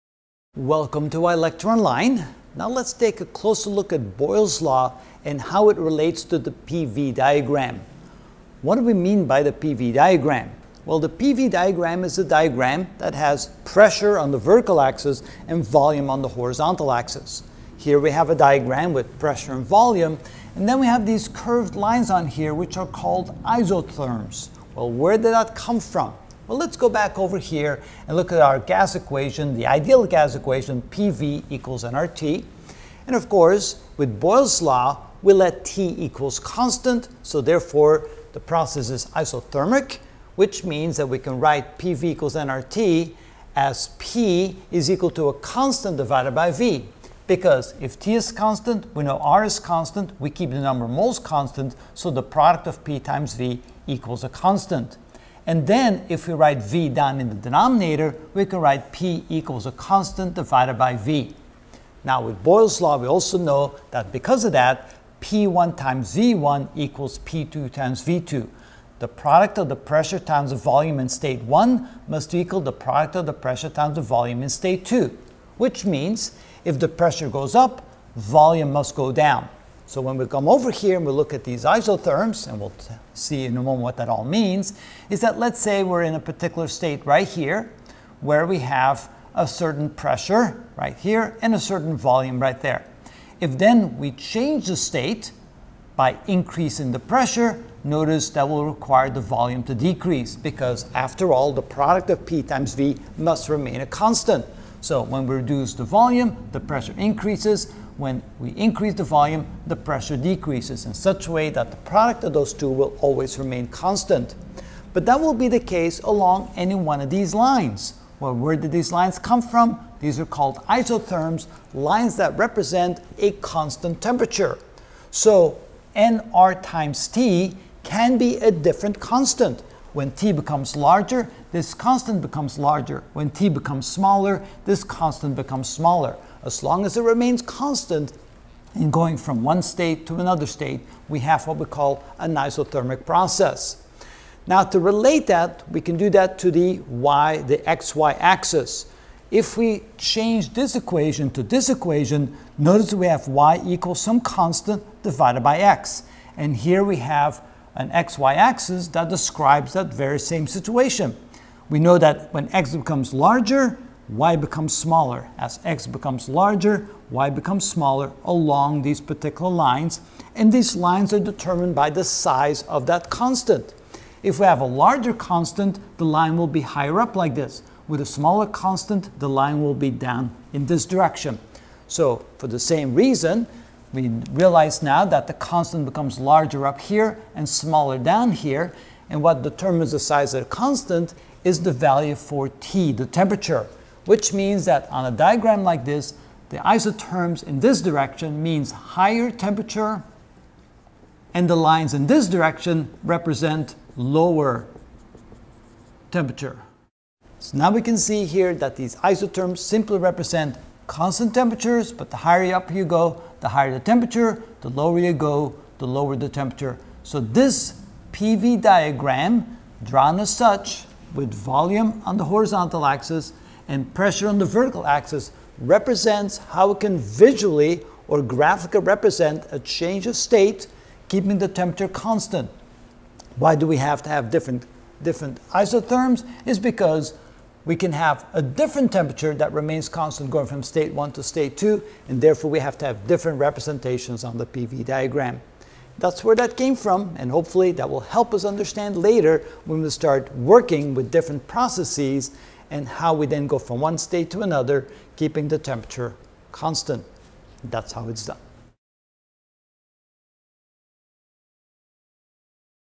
Physics - Thermodynamics 2: Ch 32.1 Def. and Terms (10 of 23) Boyle's Law
In this video I will give and explain Boyle's Law and P1V1=P2V2.